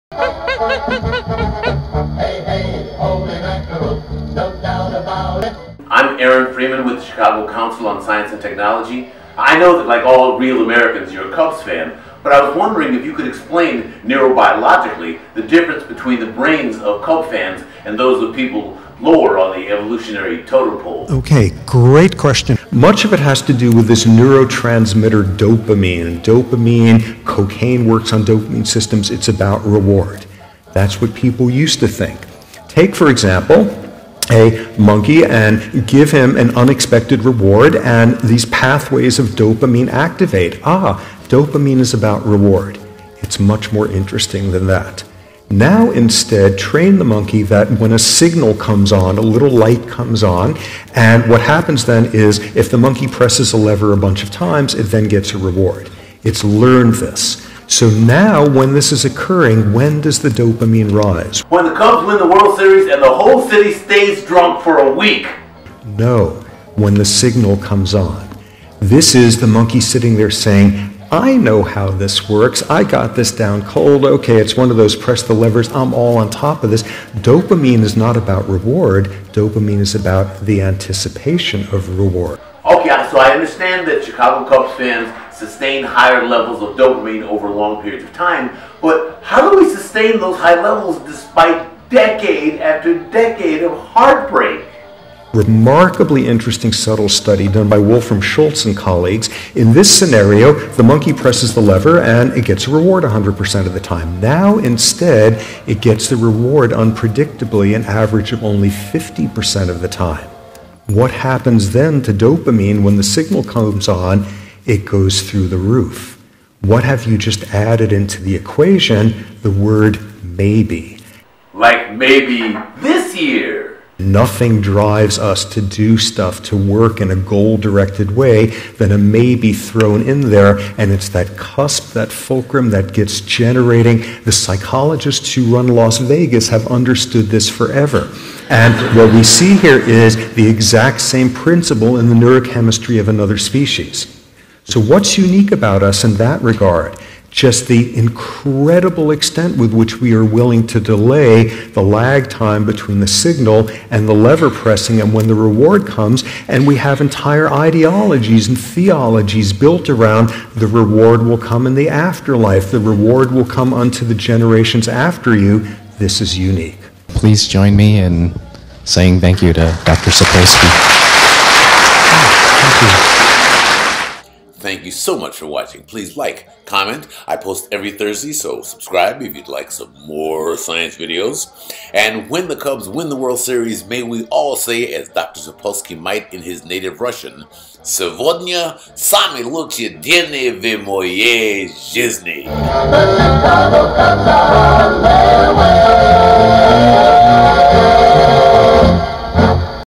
Neuroscience of Chicago Cubs Fans
C2ST Artist in Residence Aaron Freeman pretends to interview Stanford University Neurobiology professor Robert Sapolsky on the difference between the brains of Chicago Cubs fans and those of lesser beings.  According to Sapolsky part of the difference may have  to do with higher sustained levels of the neurotransmitter dopamine.